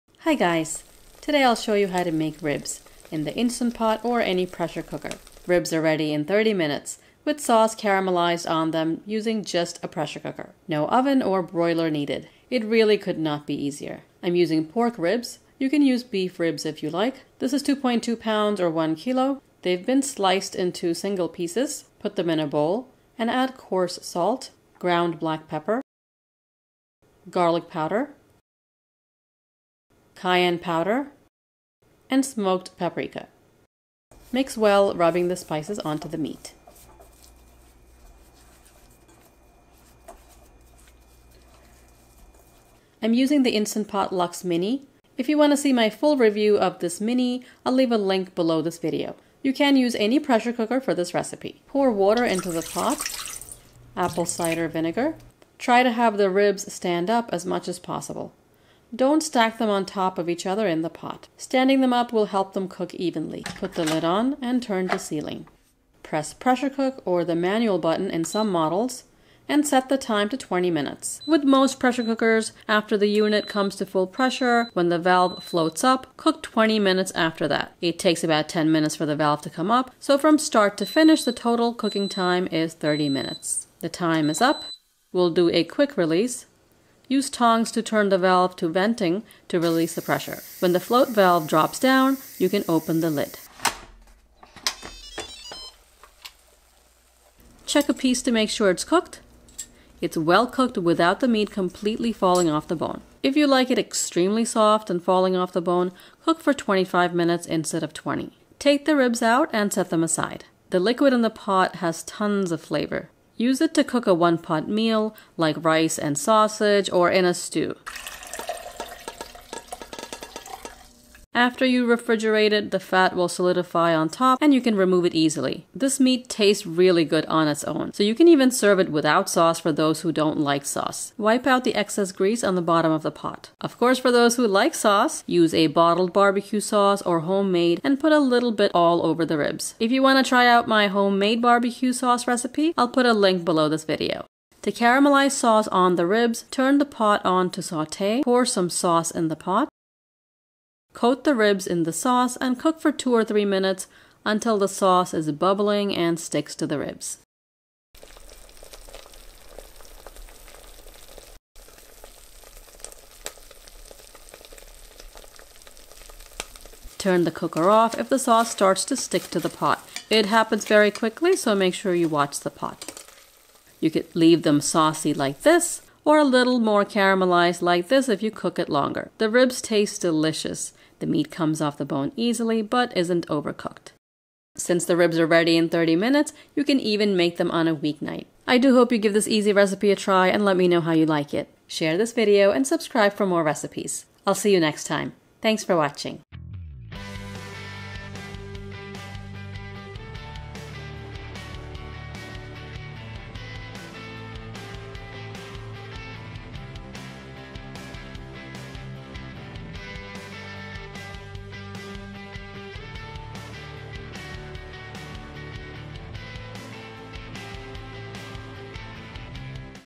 Instant Pot Ribs Recipe
Hi Guys, today I’ll show you How to Make Ribs in the Instant Pot or any pressure cooker. Ribs are ready in 30 minutes with sauce caramelized on them using just a pressure cooker! No Oven or Broiler needed! Really could not be easier. RECIPE
LINK to Instant Pot Mini Lux
LINk to Instant Pot Mini Duo

I’m using pork ribs, you can use beef ribs if you like. 2.2lbs or 1 Kilo. They’ve been sliced into single pieces. Put them in a bowl and add coarse salt, ground black pepper, garlic powder, ½ tsp cayenne powder and smoked paprika. Mix well rubbing the spices onto the meat. I’m using the Instant Pot Lux Mini. I have done a full review of this Mini, if you want to see it, I’ll leave a link below this video. You can use any pressure cooker for this recipe. Pour water into the pot. Apple Cider Vinegar. Try to have the ribs stand up as much as possible, don’t stack them on top of each other in the pot. This will help them cook evenly. Put the lid on and turn to sealing. Press pressure cook or the manual button in some models and set the time to 20 mins. For most pressure cookers, after the unit comes to full pressure, when the valve floats up, cook 20 mins after that. It takes about 10 minutes for the valve to come up, so from start to finish, the total time is 30 minutes. Time is up, we’ll do a quick release. Use tongs to turn the valve to venting to release the pressure. When the float valve drops down, open the lid. Check a piece to make sure it’s cooked. It’s well cooked without the meat completely falling off the bone. If you like it extremely soft and falling off the bone, cook for 25 minutes instead of 20. Take the ribs out and set aside. This liquid has tons of flavor, use it to cook a one pot meal like rice and sausage or in a stew. After you refrigerate it, the fat will solidify on top and you can remove it easily. Wipe out the excess grease on the bottom of the pot. This meat tastes really good on it’s own, so you can even serve it without sauce for those who don’t like sauce. For those who like sauce, use a bottled bbq sauce or homemade and put a little bit all over the ribs. I’ll put a link to my homemade bbq sauce below the video if you want to try it. To caramelize sauce on the ribs, turn the pot on to saute. Pour some sauce in the pot. Coat the ribs in sauce and cook for 2 or 3 mins until the sauce is bubbling and sticks to the ribs. Turn the cooker off if the sauce starts to stick to the pot. It happens very quickly. Leave them saucy like this or a little more caramelized like this if you cook it longer. The ribs taste delicious, the meat comes off the bone easily but isn’t overcooked.
I hope you give this easy recipe a try and let me know how you like it.